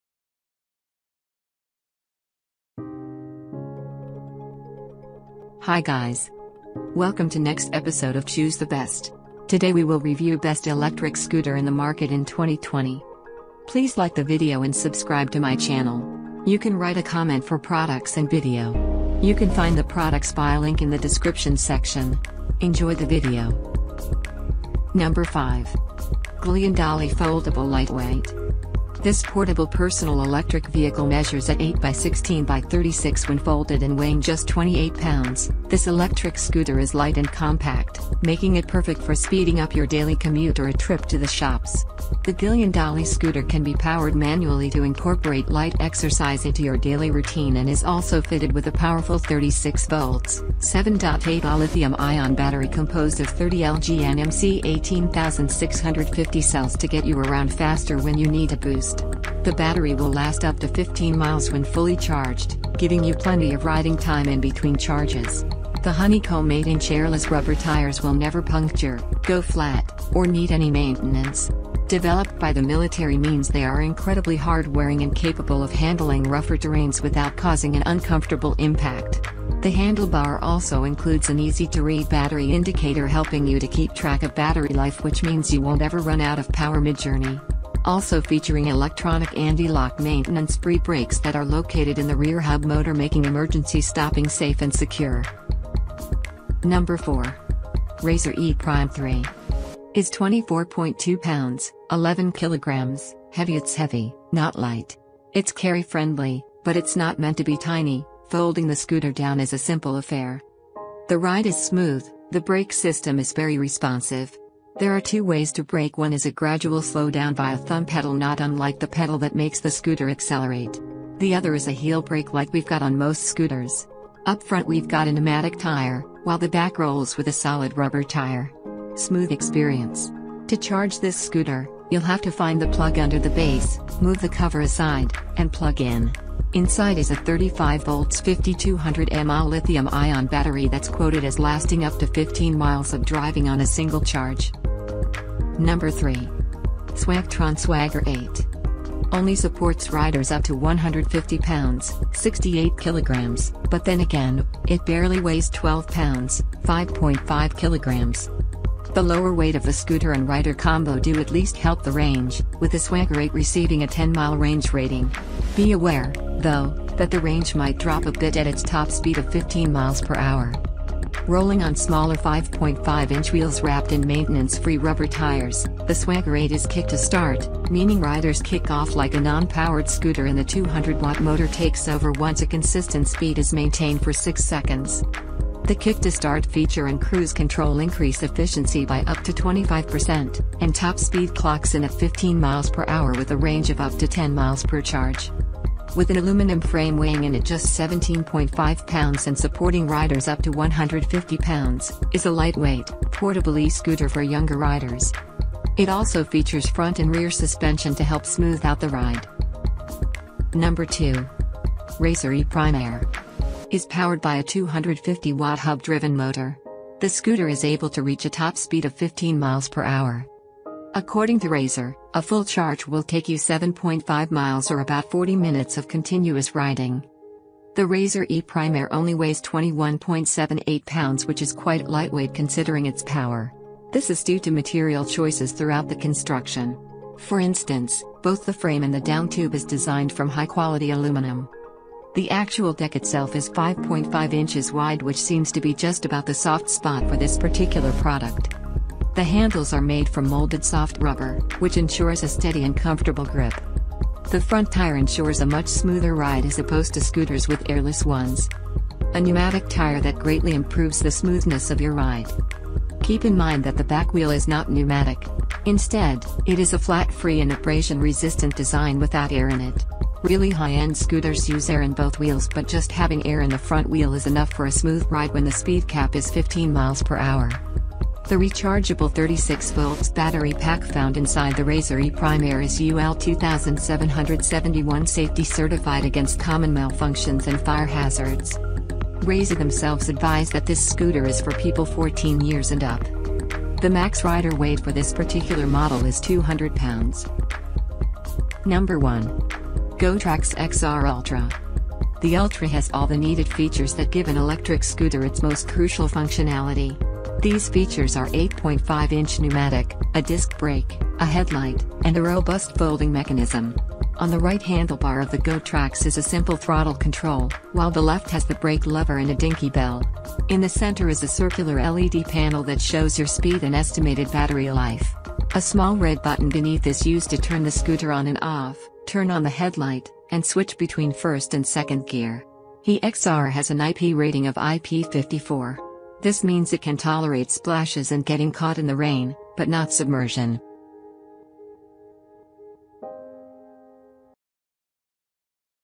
BEST ELECTRIC SCOOTER
1-GoTrax XR Ultra Electric Scooter

2-Razor E Prime Air Electric Scooter

3-Swagtron Swagger 8 Electric Scooter

4-Razor E Prime III Electric Scooter

5-Glion Dolly Foldable Lightweight Adult Electric Scooter

In this video, i listed the top 5 best electric scooter in the market in 2020.  I made this list based on my personal opinion, and i tried to list them based on their price, durability, quality and more..

Welcome to Choose The Best